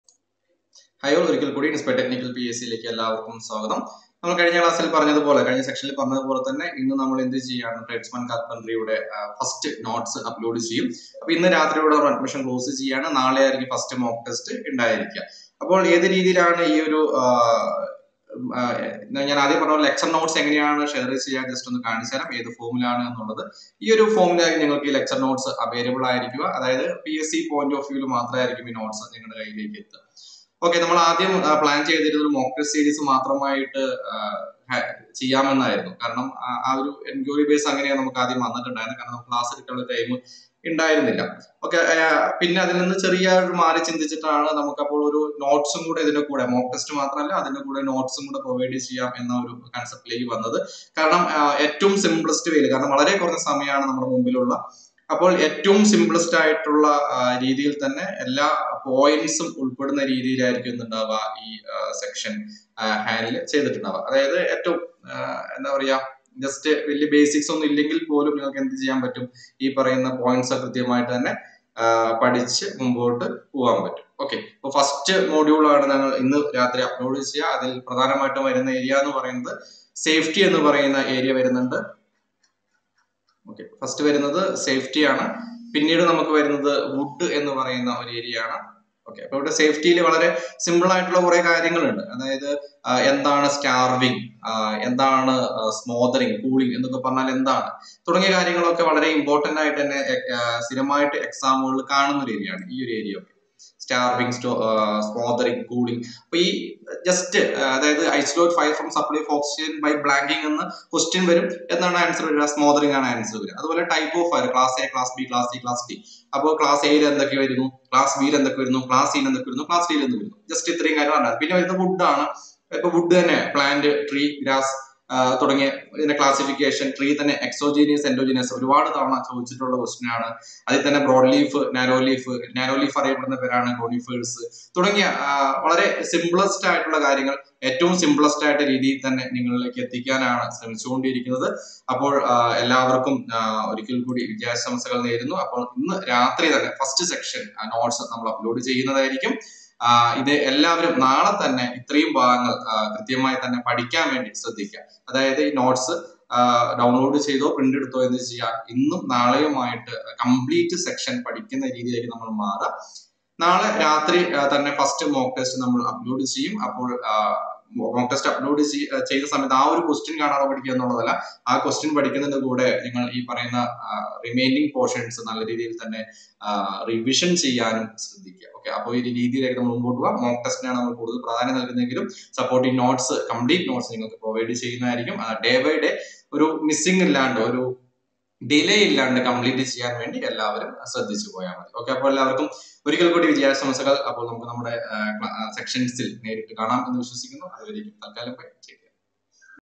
TRADESMAN CARPENTER|Preparation Strategy
Hi all..
April മാസം 9 നു നടക്കുന്ന Tradesman Carpenter പരീക്ഷക്ക്‌ വേണ്ടി ഒരു special guidence course മാർച്ച്‌ 15 മുതൽ തുടങ്ങുകയാണ്...

20 Day Special Guidence Course For Tradesman Carpenter Exam Just @299

Course Procedure:
**Will provide Lecture Notes on daily Basis(Important point will highlight)
**Daily Mock test based on the Lectur Notes

താൽപര്യം ഉള്ളവർക്ക് താഴെ കാണുന്ന link വഴി admission എടുക്കാവുന്നതാണ്..

Note:
special Guidence കോഴ്സ് ആയതു കൊണ്ട് തന്നെ മാർച്ച്‌ 14 നു അഡ്മിഷൻ closs ചെയ്യുന്നതായിരിക്കും...